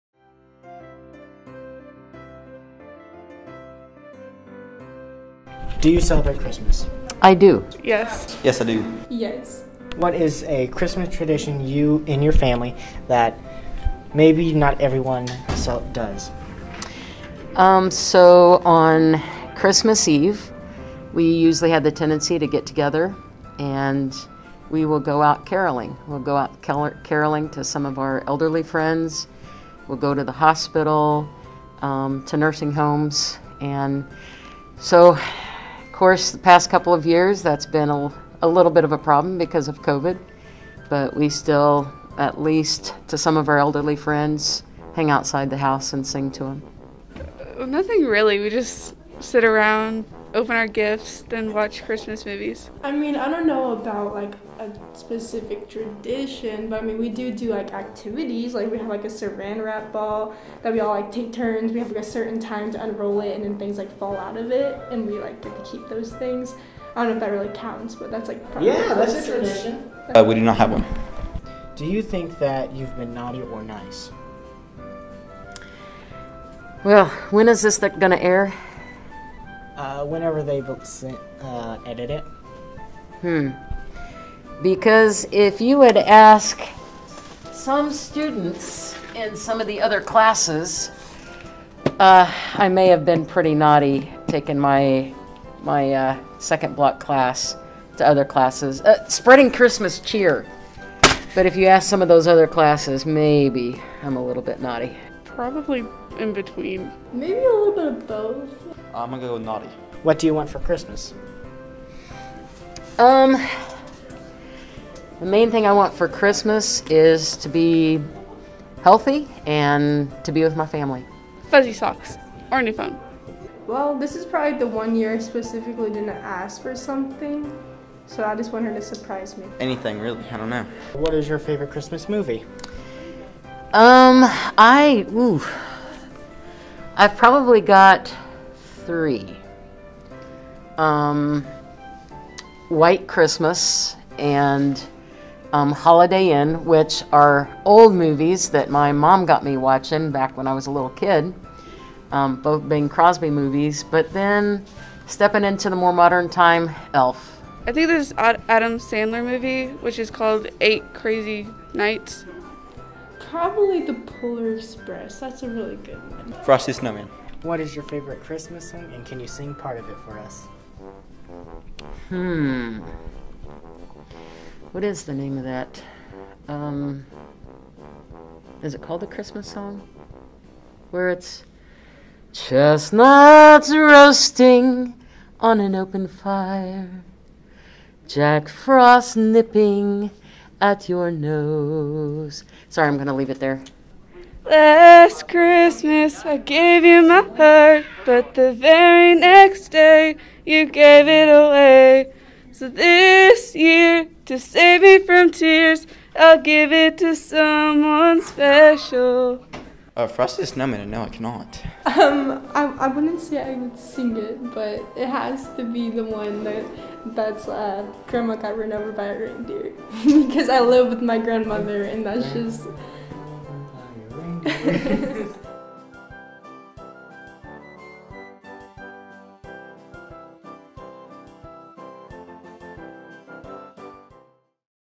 Christmas at RCHS - Roaming the Halls
The VAT Crew roams the halls of RCHS before Christmas in search of cheer and ANSWERS!